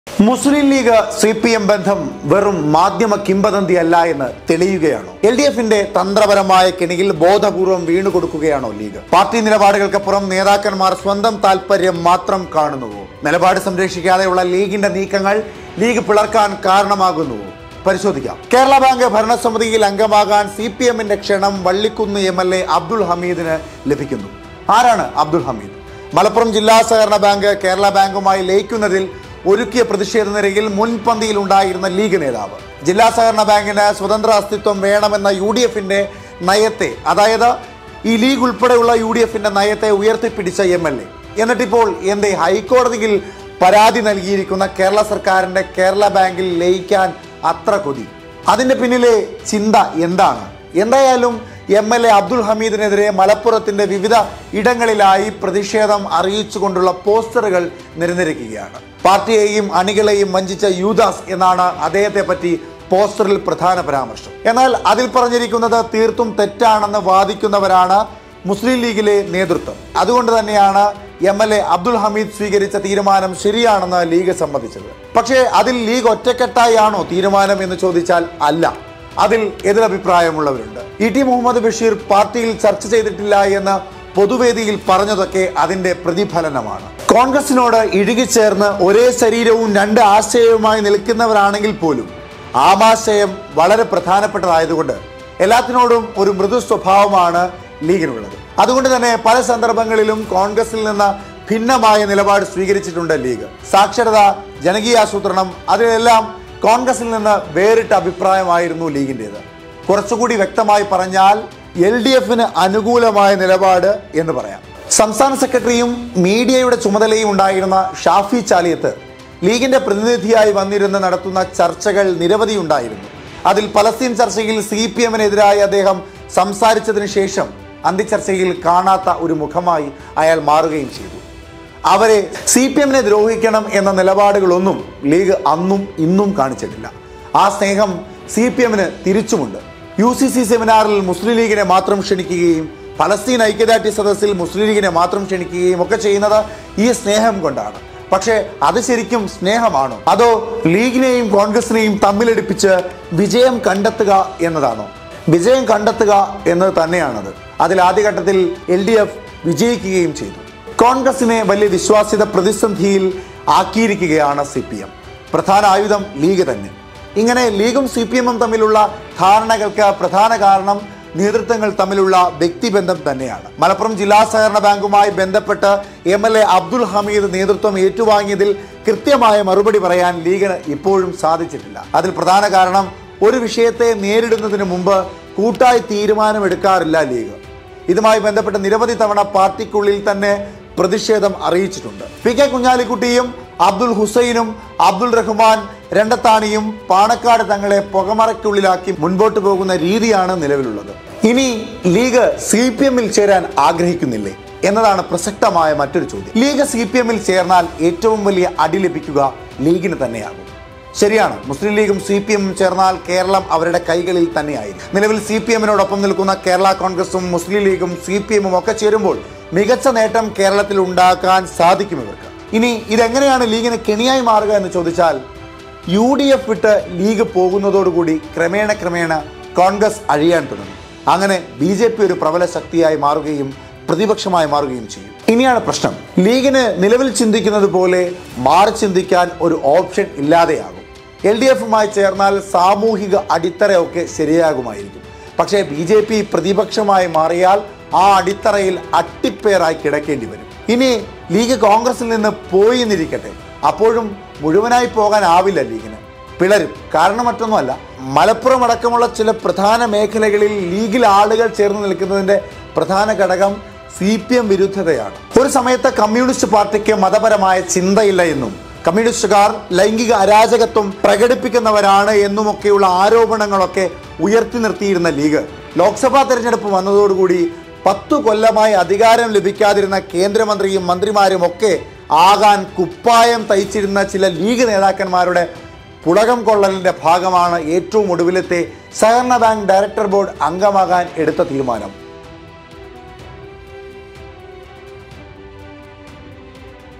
ലീഗ് ലോക്ക് ആവുകയാണോ? | Muslim League | CPM | Congress | M5 News
കോൺഗ്രസ് വിടാനുള്ള ലീഗിന്റെ തീരുമാനം അവർക്ക് വിനയോ വിളവോ?